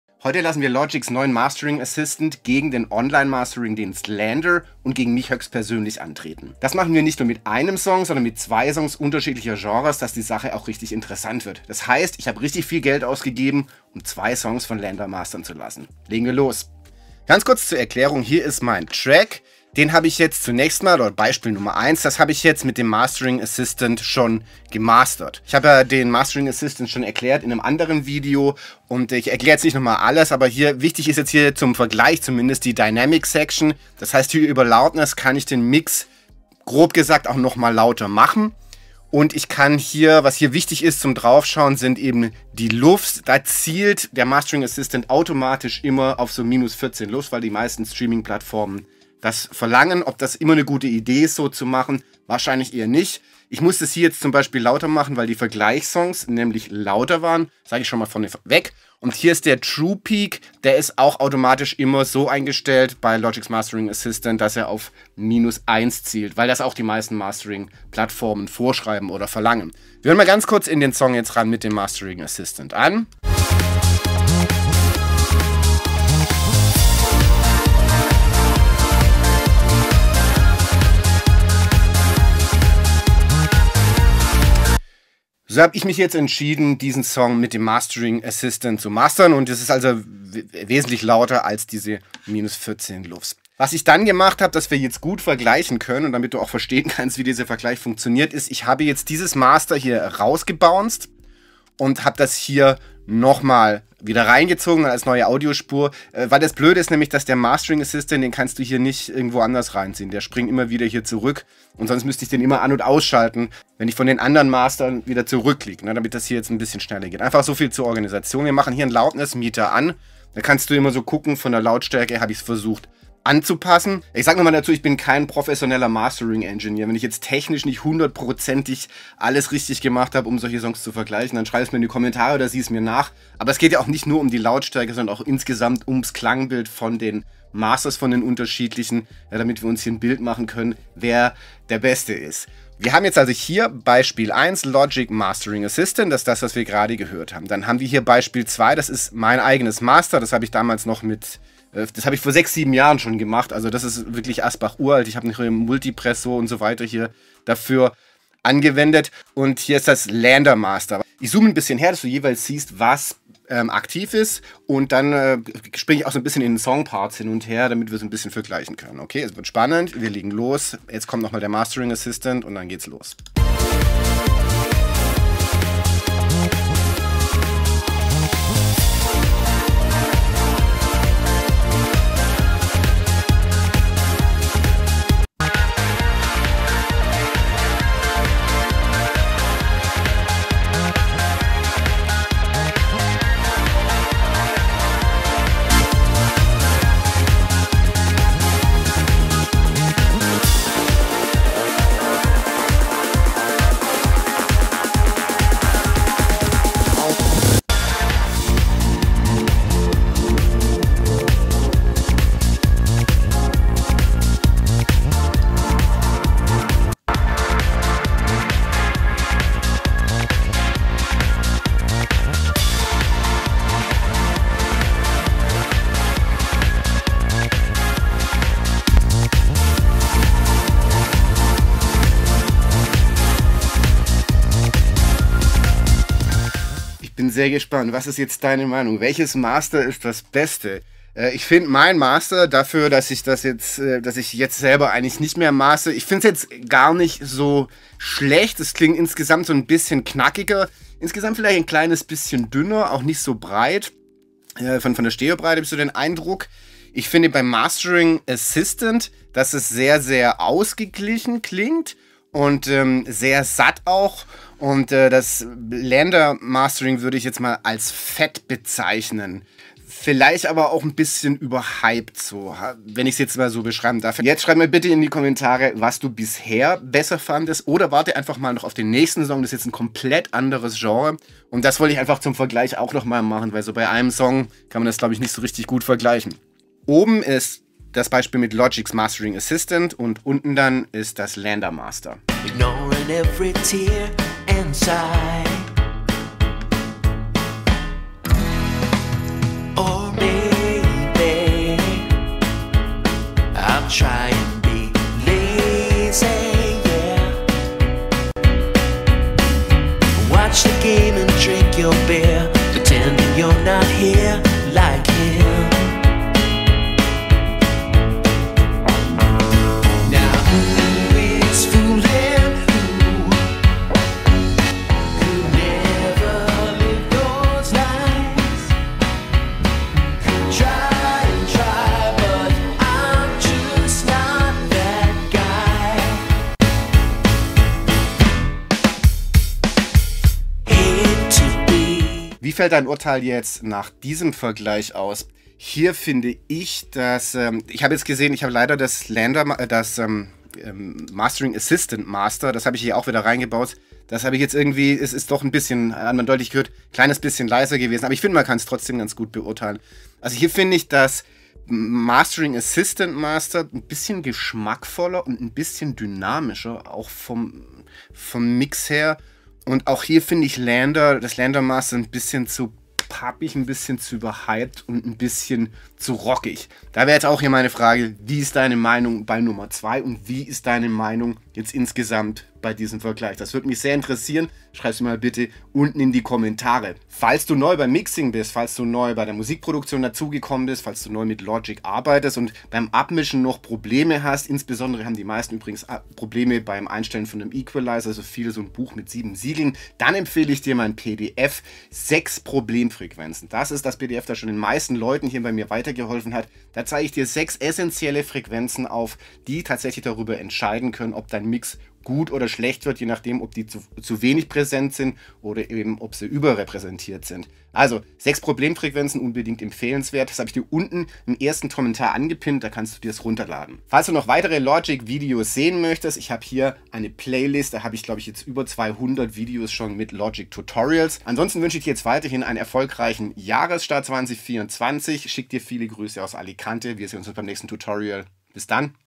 Welches KI Tool ist besser? (Mastering Assistant Logic Pro X vs LANDR)
Heute machen wir den direkten Vergleich: LANDR vs Logic`s Mastering Assistant vs mich! Welche KI Software klingt besser? Beim Mixing uns Audio Engineering haben wir in der Version Logic Pro X 10.8 ein neues Mastering Plugin mit geschenkt bekommen. Wie schneidet es im Vergleich zu anderen Tools dieser Art ab?